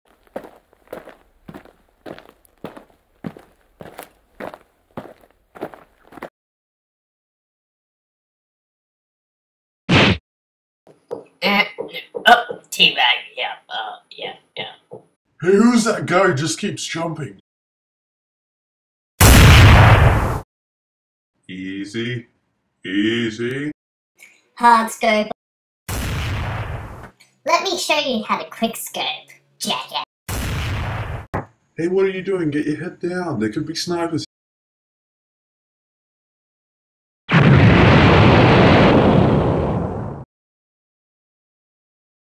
If noobs got real guns
Yeah just thinking about noobs in the fps genre made me feel like doing a video hope you enjoy and check out some others